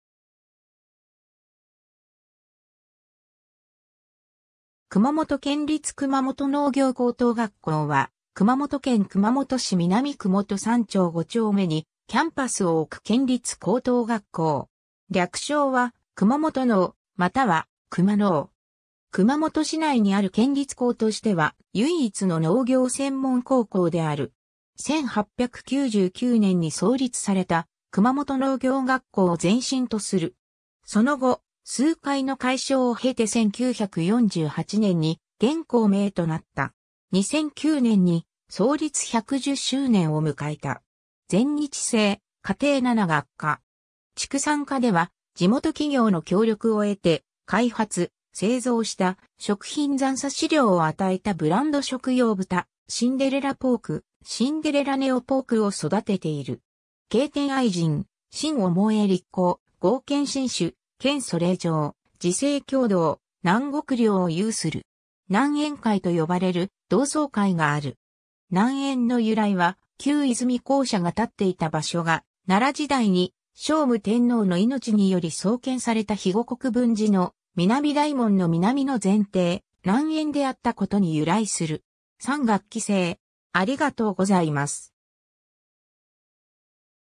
熊本県立熊本農業高等学校
熊本県立熊本農業高等学校（くまもとけんりつ くまもとのうぎょうこうとうがっこう, Kumamoto Prefectural Kumamoto Agricultural High School）は、熊本県熊本市南区元三町五丁目にキャンパスを置く県立高等学校。
略称は「熊本農」または「熊農」（くまのう）。
 熊本市内にある県立校としては唯一の農業専門高校である。
 1899年（明治32年）に創立された「熊本農業学校」を前身とする。
その後、数回の改称を経て1948年（昭和23年）に現校名となった。
2009年（平成21年）に創立110周年を迎えた。
 全日制課程 7学科（農業科、園芸・果樹科、畜産科、生活科、農業経済科、食品工業科、農業土木科）。
 畜産科では、地元企業の協力を得て開発・製造した食品残渣飼料を与えたブランド食用豚「シンデレラポーク」「シンデレラネオポーク」を育てている。
 「敬天愛人」 「愼思力行」「剛健進取」「倹素礼譲」「自制協同」 「南国寮」を有する。
 {{南園会と呼ばれる同窓会がある。
 「南園」の由来は、旧出水校舎が建っていた場所が、奈良時代に聖武天皇の命により創建された肥後国分寺の南大門の南の前庭、南苑（園）であったことに由来する。
}} 3学期制